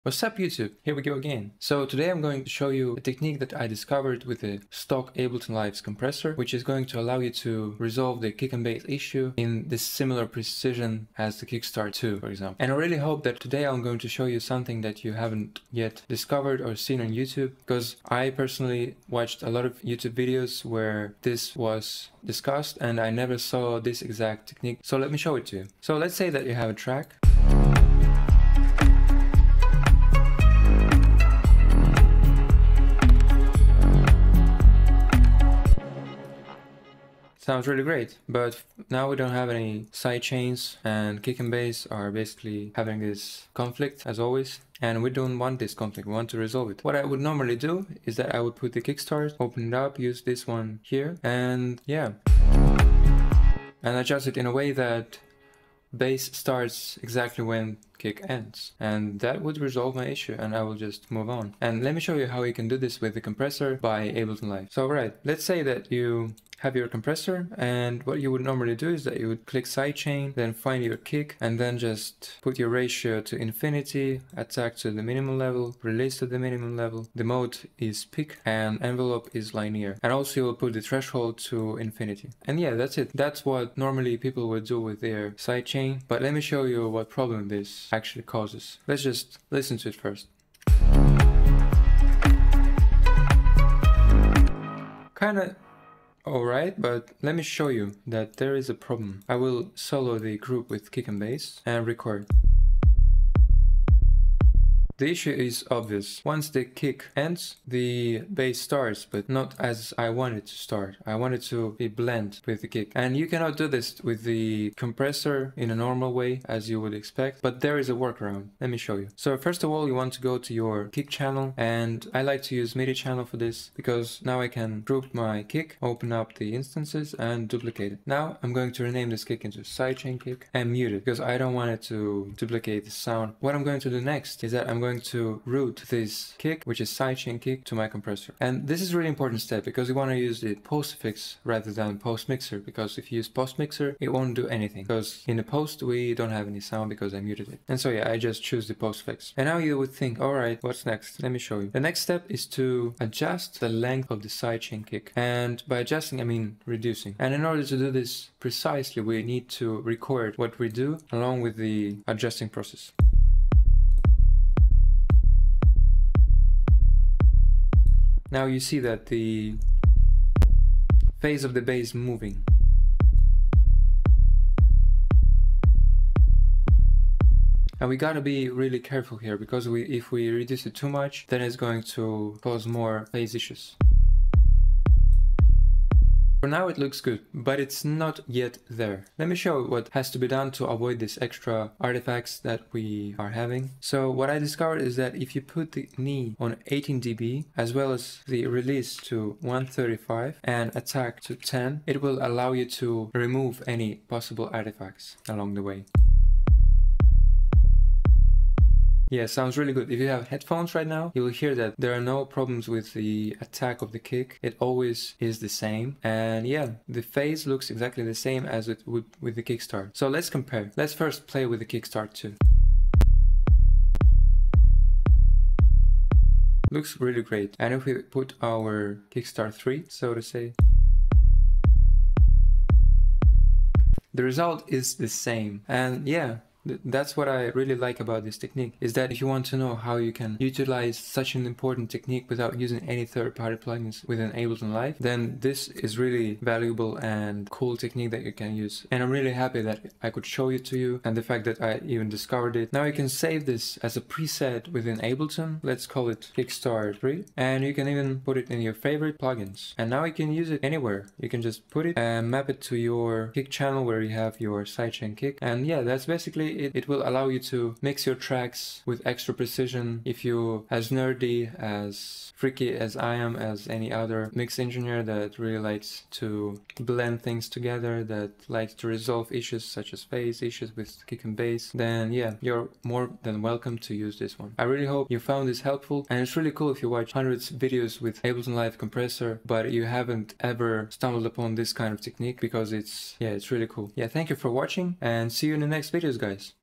Sidechain TRICK for KICK & BASS You Don't Wanna Miss | Ableton Live
In this video, I show how you can build your very own precise sidechain plugin for kick and bass in Ableton Live and how you can remove the artefacts to get similar to a Kickstart 2 VST quality result.

If your kind soul wants to support me :)

Timecodes:

00:00 - Intro
00:25 - Normal approach
01:01 - Live's Compressor technique
01:26 - Critical problem it causes
01:57 - Hidden workaround
02:18 - IMPORTANT! Step
03:05 - Extra precision method
03:39 - Comparison: Kickstart 2 vs Kickstart 3
03:55 - The Value of this technique
04:12 - Remember to Save it!
04:44 - Outro